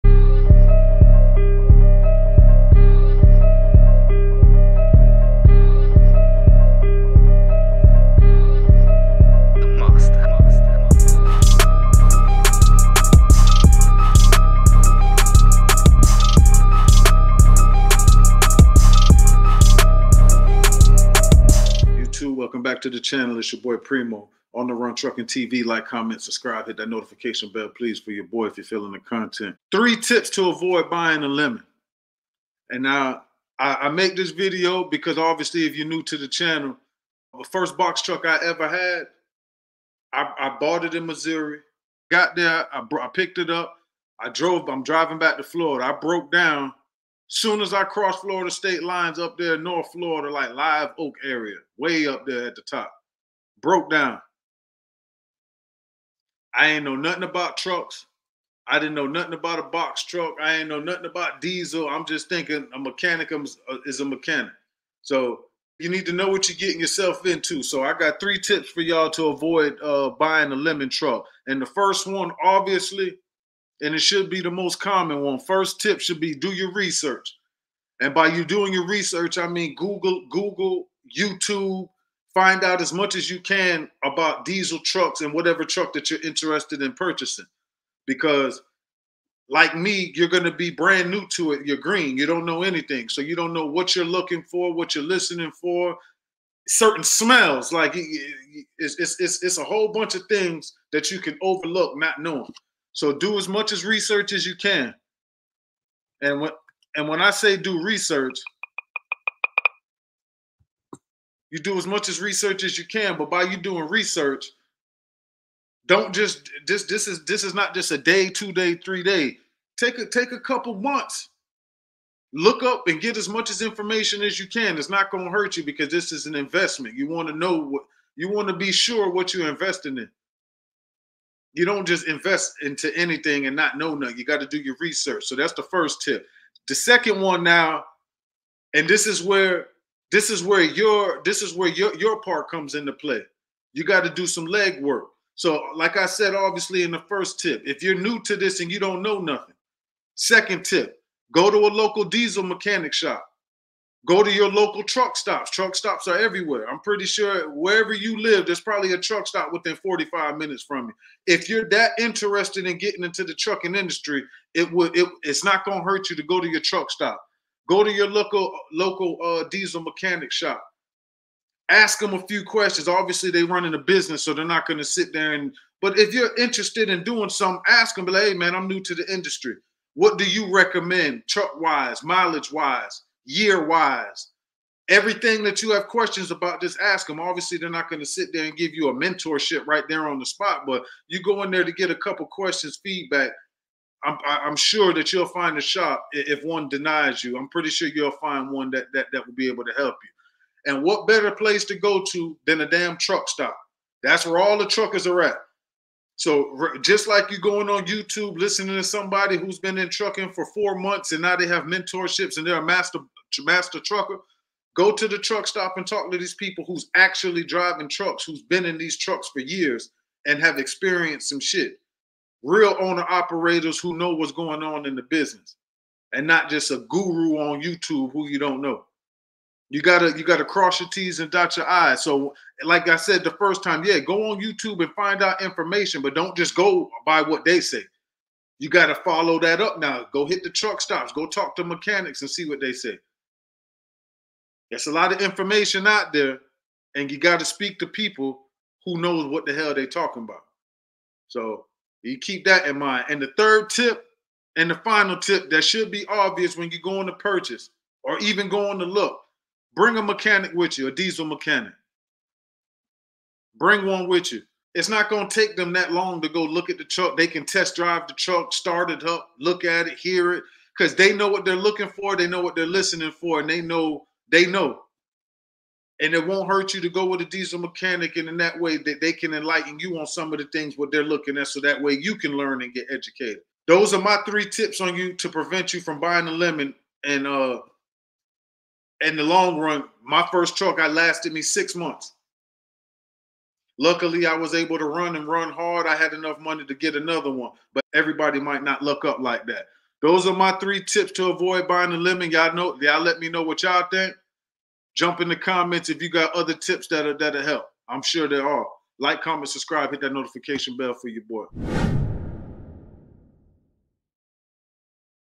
STOP WASTING MONEY ON JUNK: 3 Tips to Avoid buying a Lemon Semi Truck
*SEND ALL TRUCKING VIDEOS TO*